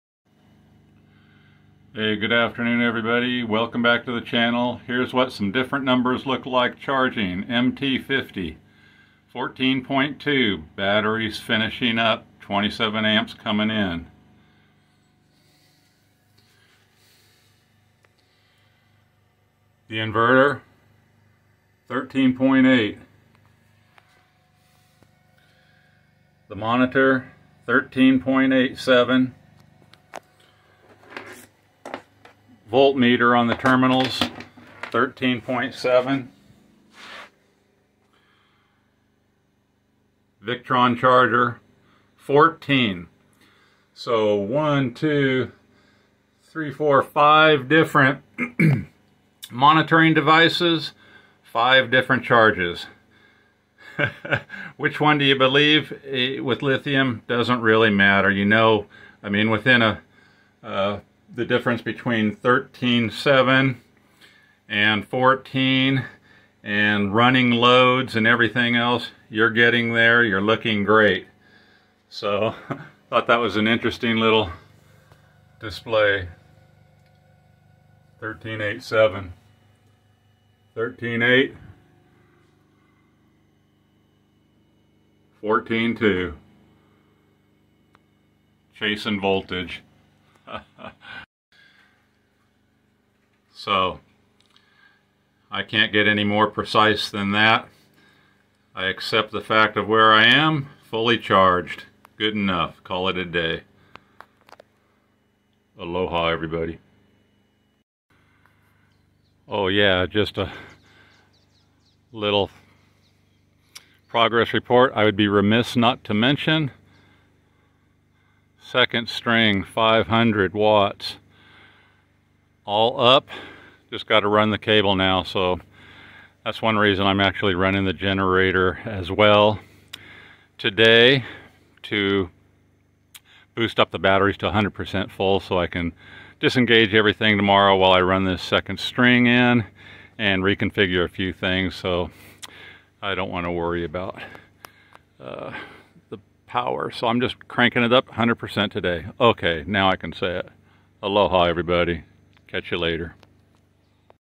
Five different voltages on five devices... close enough
Chasing voltages on five different solar monitors. Don't sweat the discrepancies.

Epever 12v 40amp controller
Victron 12v 30amp smart charger
Aili battery monitor
Newpowa 12v monocrystalline solar panel
Reliable 12v3000watt pure sine wave inverter
Chins 12.8v 300ah LiFePO4 battery